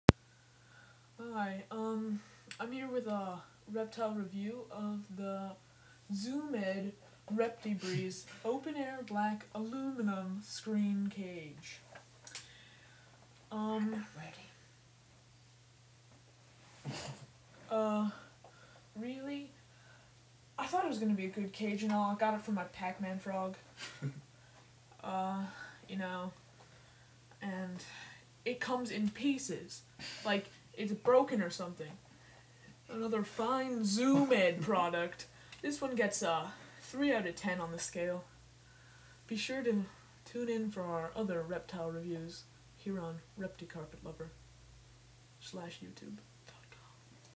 Reptile Review: ZooMed Repti-Breeze
A review of the "broken" ZooMed Product